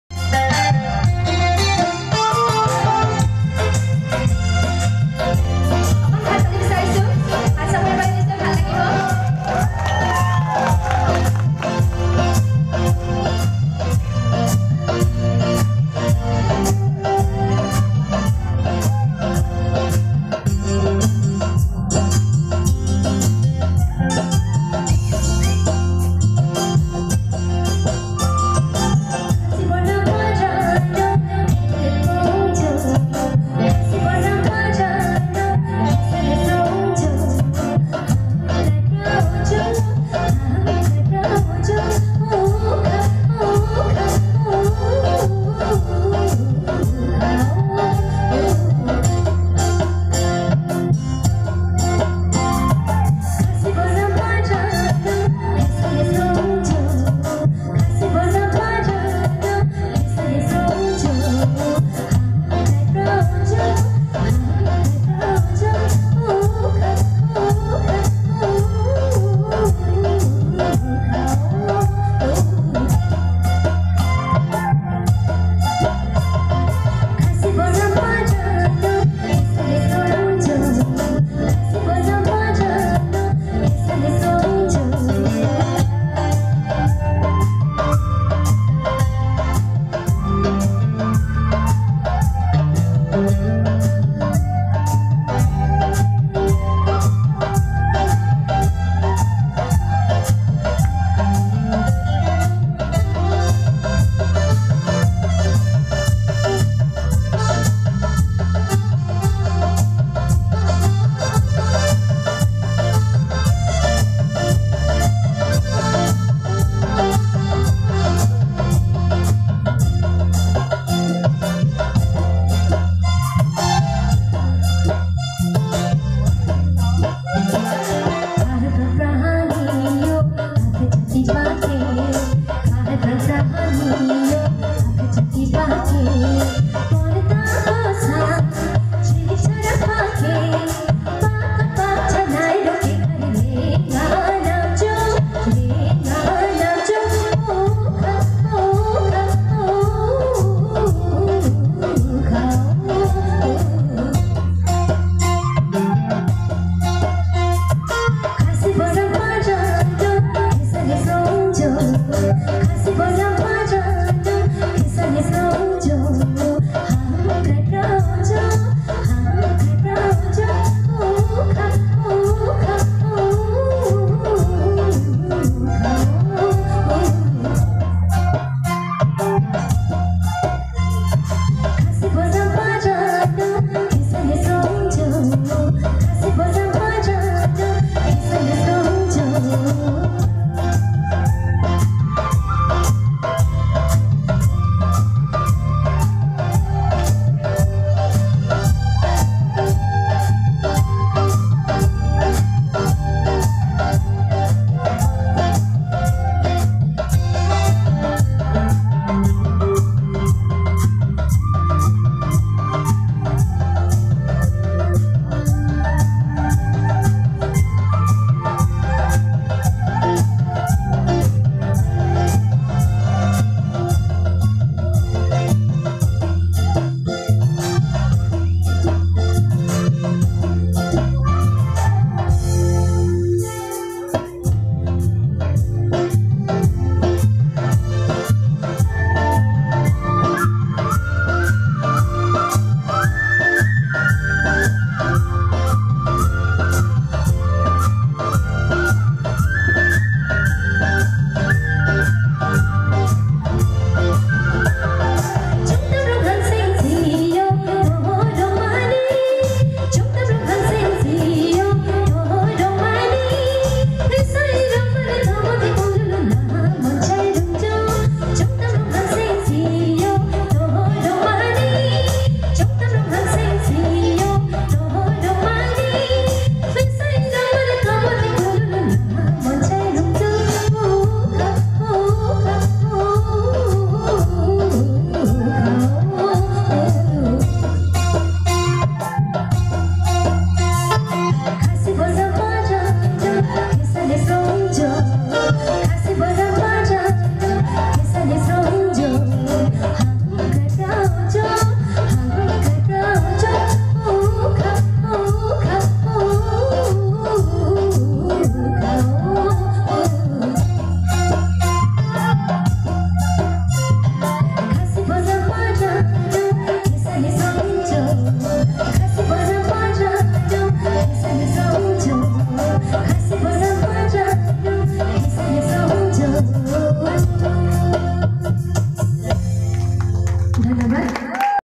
Rabha_sangeet_|| By_mery_rabha_
Rabha sangeet by Mery Rabha at Rabha divas DAIRONG